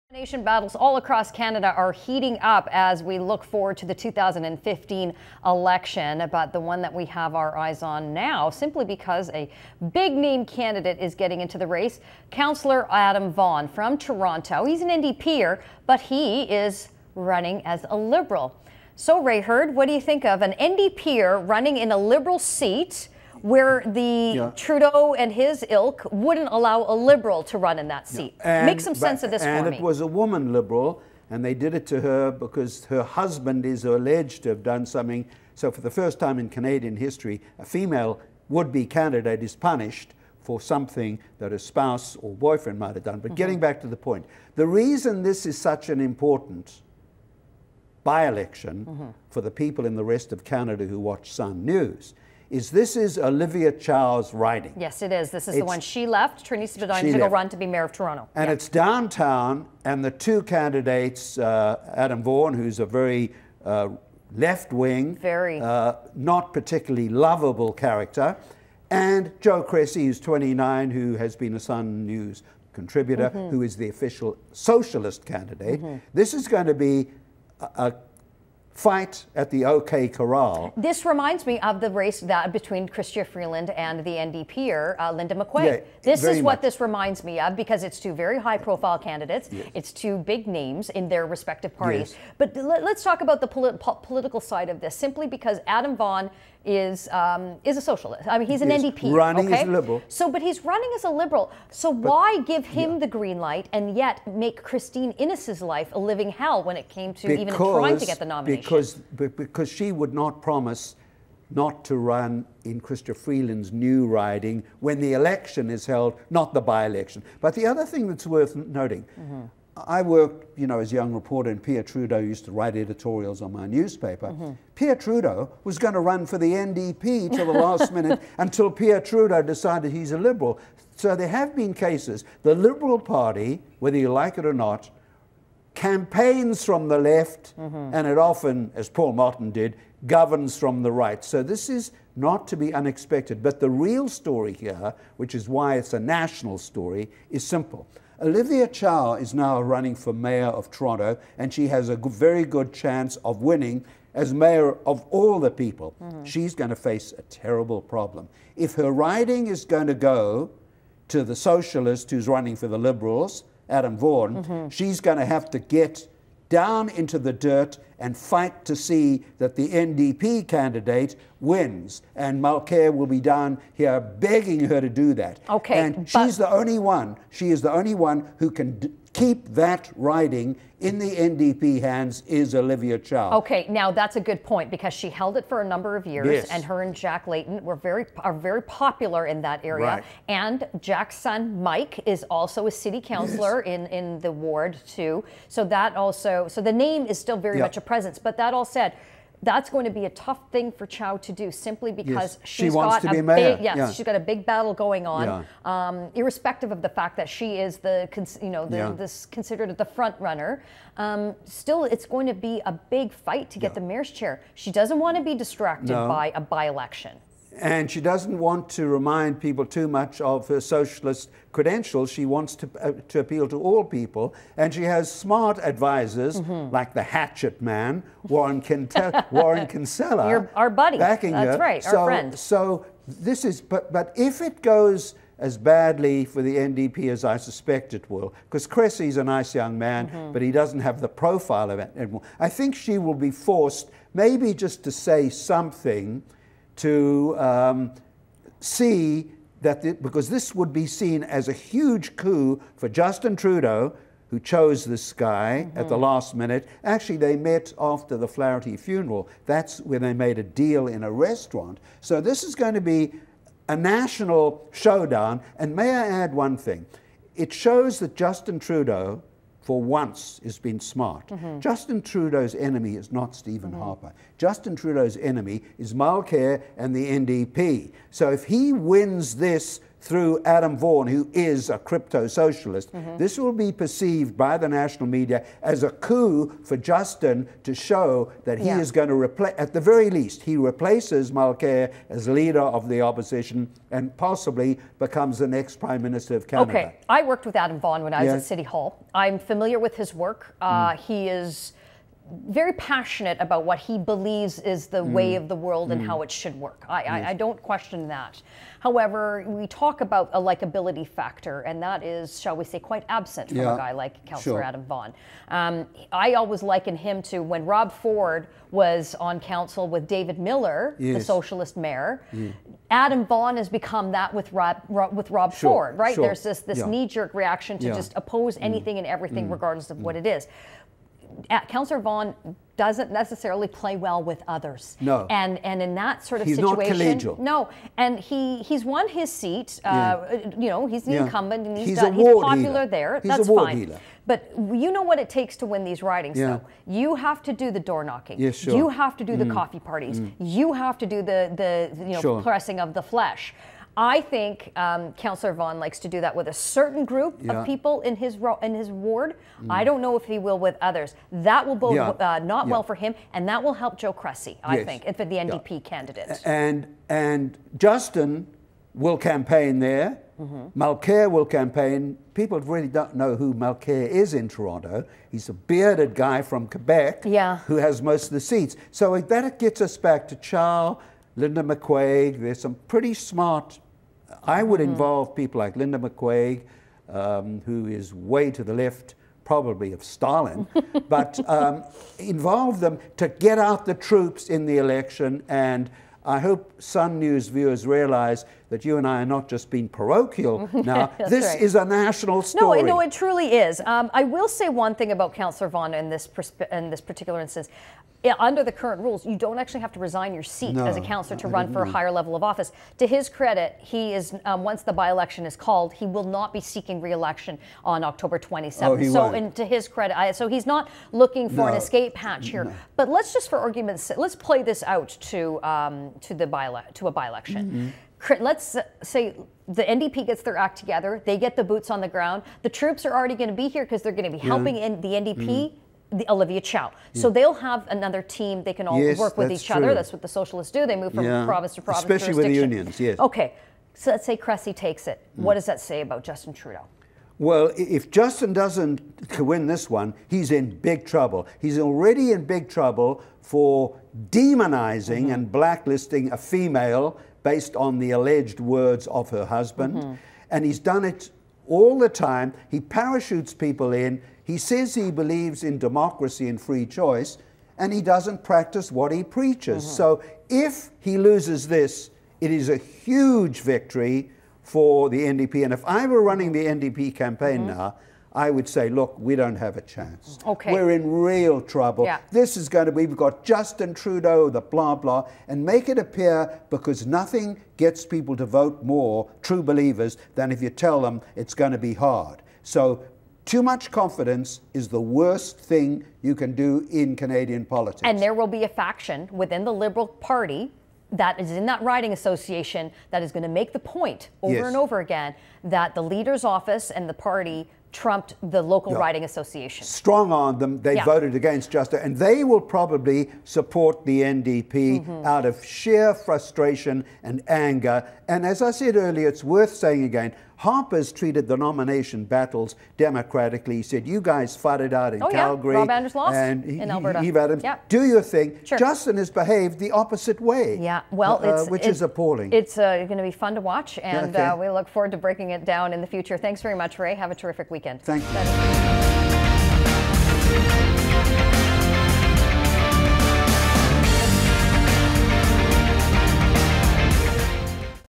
Trudeau's New Toronto Friend
Liberal Leader Justin Trudeau hand-picks Toronto city councillor Adam Vaughan as his candidate in Olivia Chow's old riding.